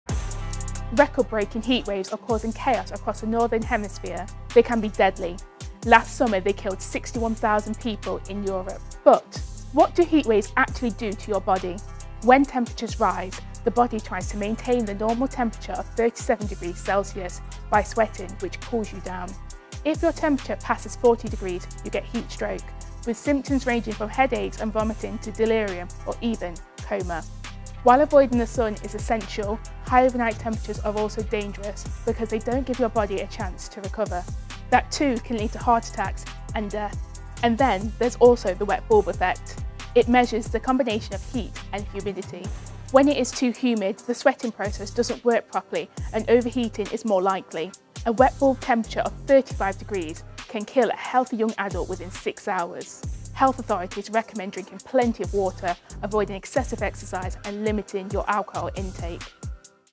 What happens to your body in a heatwave?
Record-breaking heatwaves are causing chaos across the northern hemisphere.
They can be deadly: Last summer, they killed 61,000 people in Europe.
But what do heatwaves actually do to your body?
When temperatures rise, the body tries to maintain its normal temperature of 37 degrees Celsius by sweating, which cools you down.
If your temperature passes 40 degrees, you get heatstroke, with symptoms ranging from headaches and vomiting to delirium or even coma.